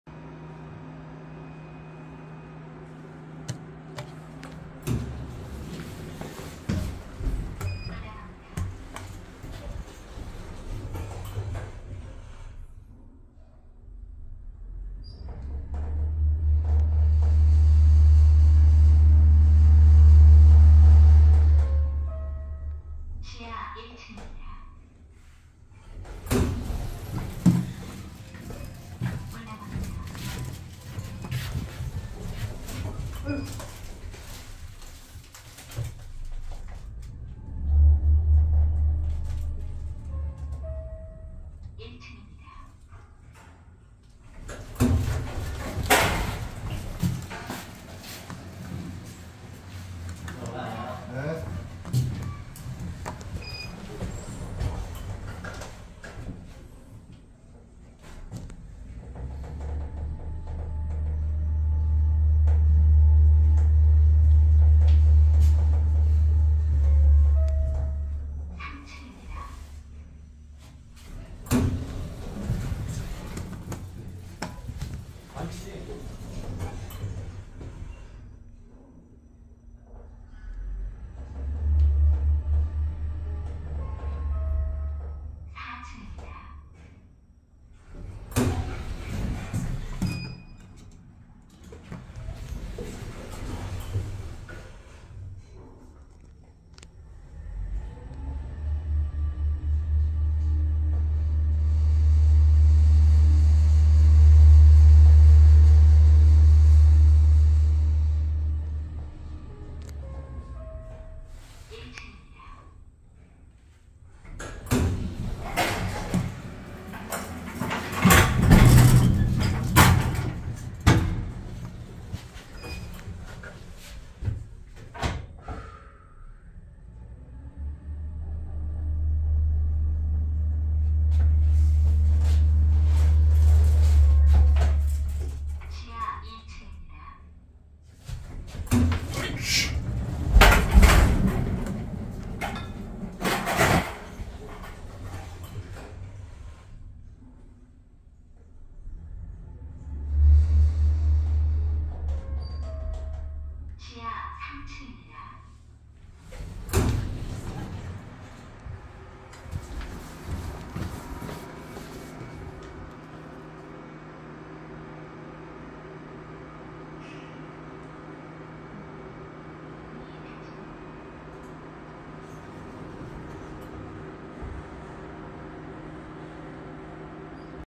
서울특별시 서초구 바우뫼로 195 (양재동) 남서울농업협동조합 OTIS엘리베이터 탑사기
원활한 탑사진행을 위해 B3층에서 시작했으며, 오르막과 내리막 도중 1층에 정차한점 양해바랍니다.

* 여기는 내부에 CCTV가 설치되어 있거나 상당히 혼잡할 수 있으니 탑사시 주의하십시오.